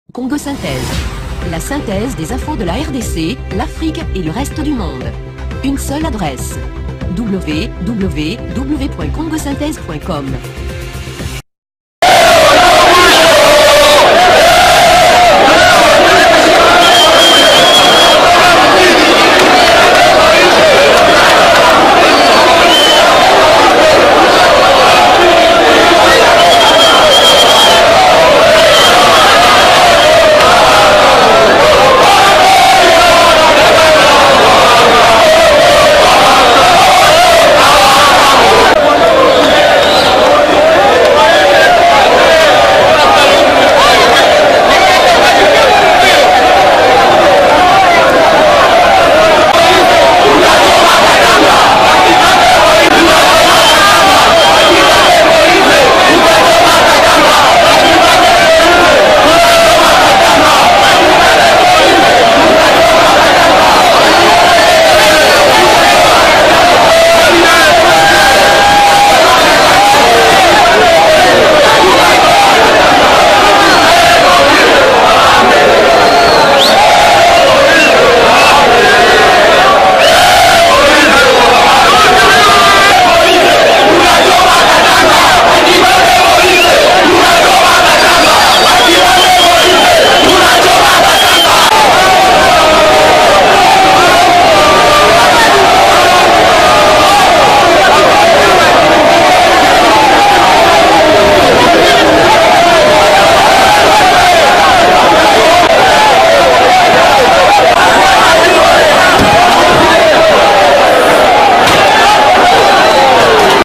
Lubumbashi: L'audition de Moise Katumbi reportée suite aux incidents.
Lubumbashi - Moise Katumbi | Audition reportée à une date ultérieure non communiquée.